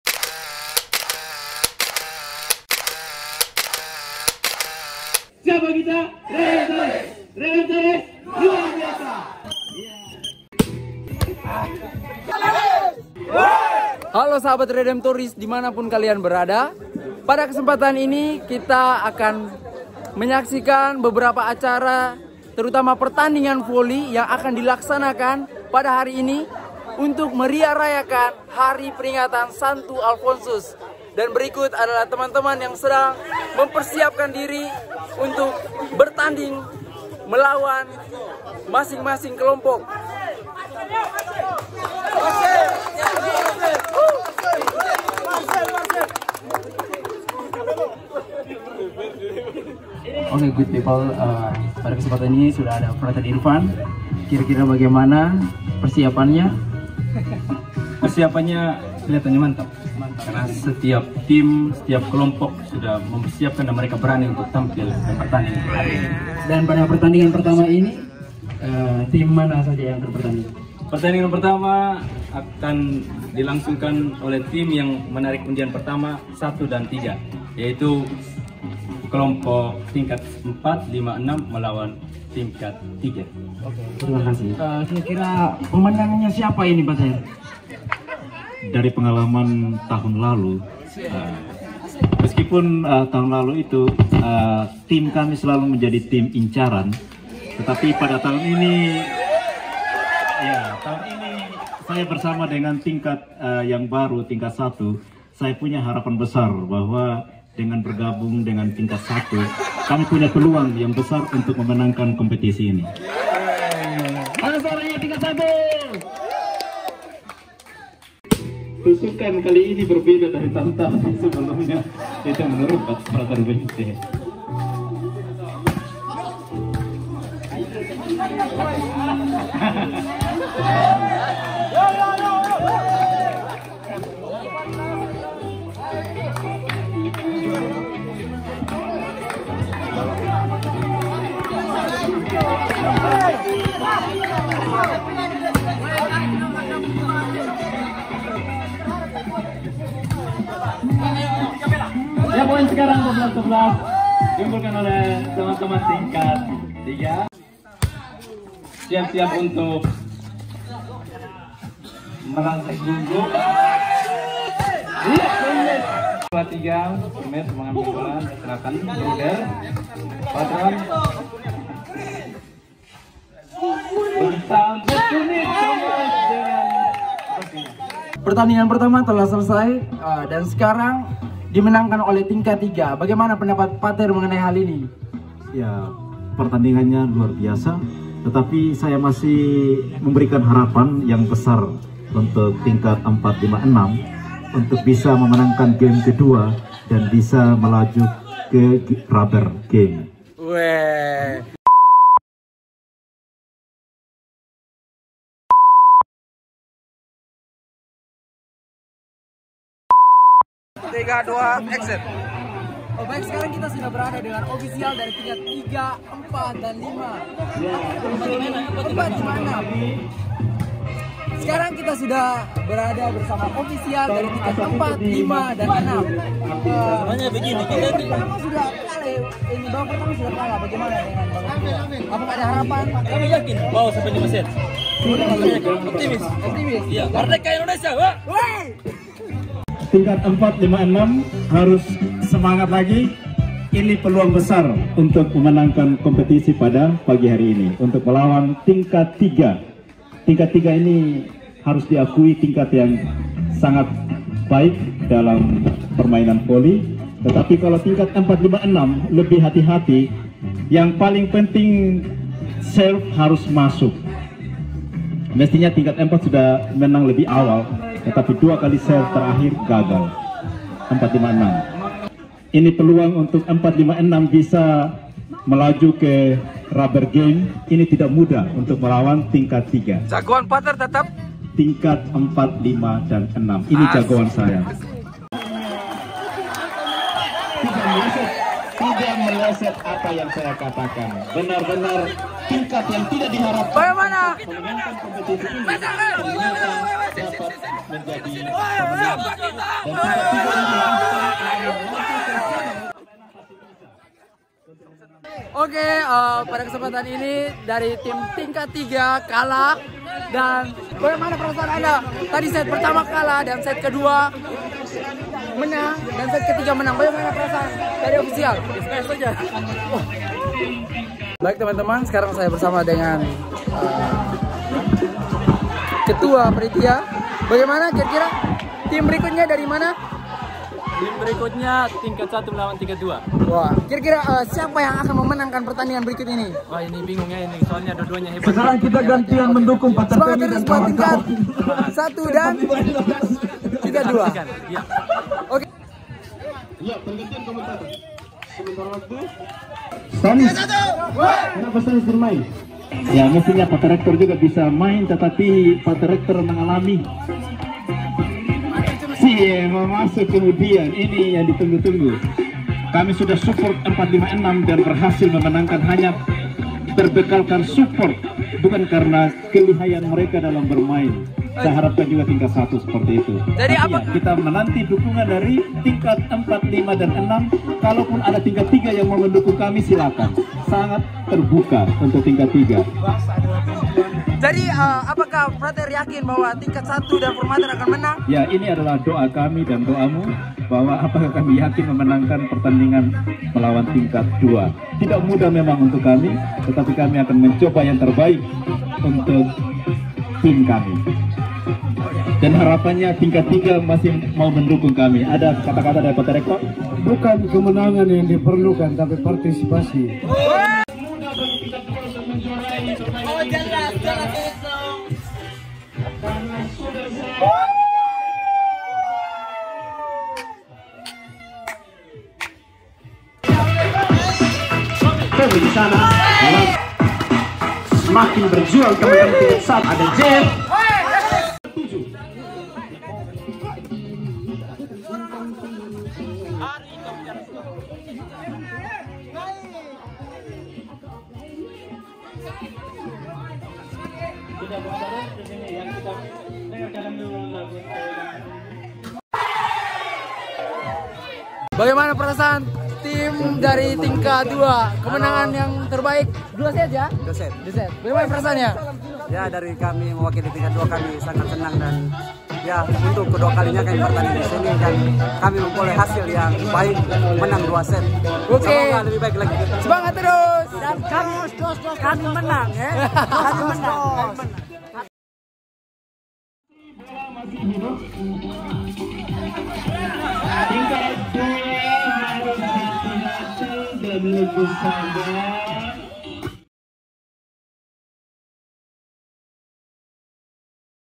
WSP Vlog Day 01 || Alfonso Cup: Memeriahkan Pesta St. Alfonsus Maria de Liguori
Alfonso Cup adalah kegiatan perlombaan para frater di Wisma Sang Penebus, dalam rangka memeriahkan dan menyambut Pesta St.Alfonsus Maria de Liguori, Pendiri Kongregasi Redemptoris pada setiap 1 Agustus.

Apa saja perlombaanya? Penasaran kan…..
Yuk saksikan keseruan kami dalam Vlog Video kali ini.

Semangat selalu dan Jah bless….

St. Alfonsus doakanlah kami.

Salam Sang Penebus….